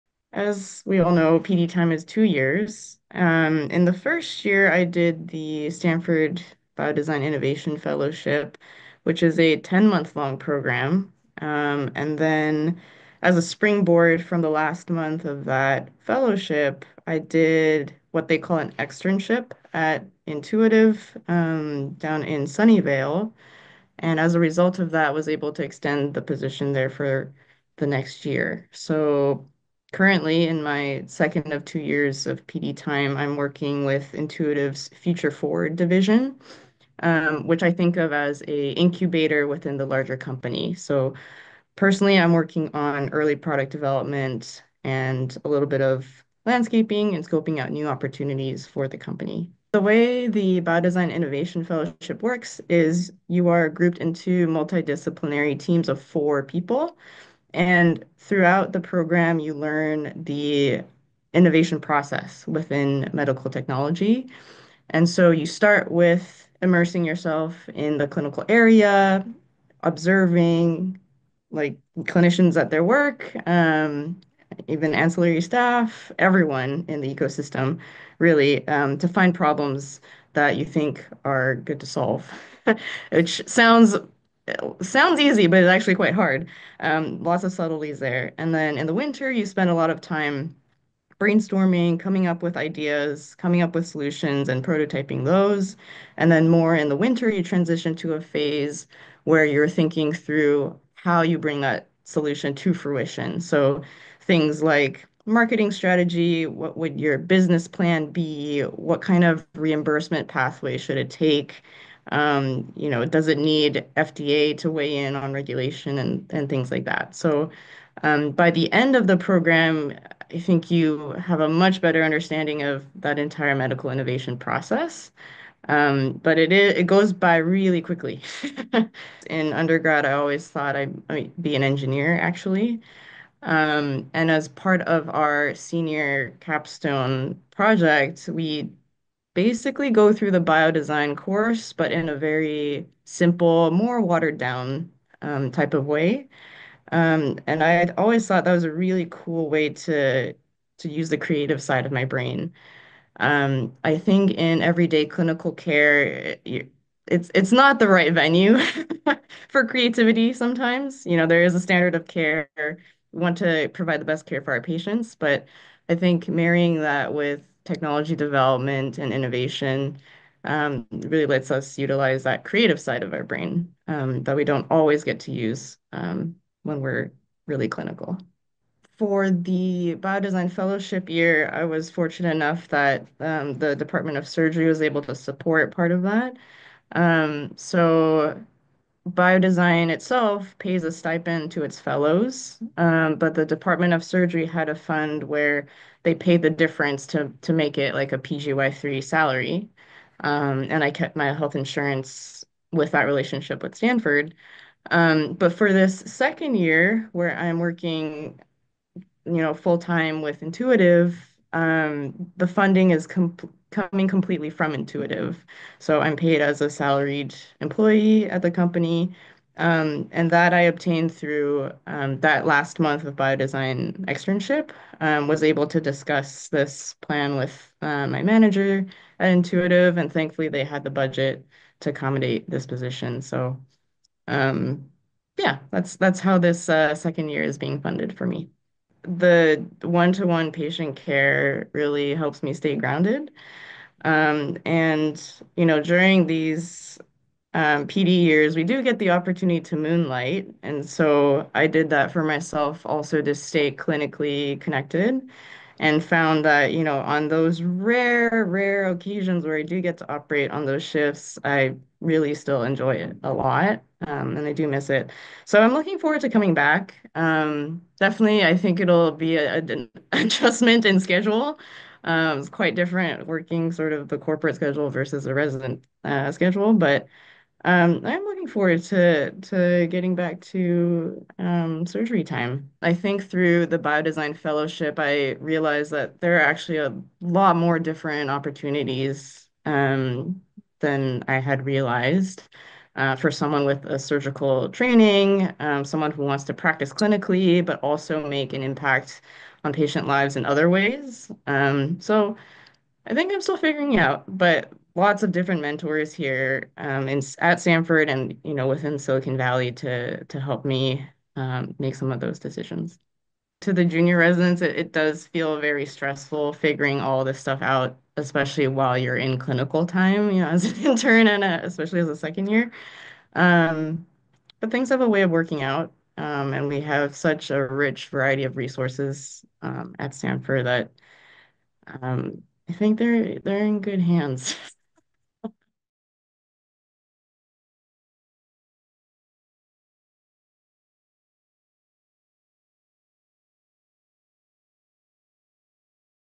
PD Interview with General Surgery Resident Dr. Kay Hung
Dr. Kay Hung, a resident in our General Surgery Residency Program, spent the first year of her professional development as a Stanford Biodesign Innovation Fellow. After an externship her final month, she was able to secure a position at Intuitive Surgical, maker of the Da Vinci Robotic system, for the second year.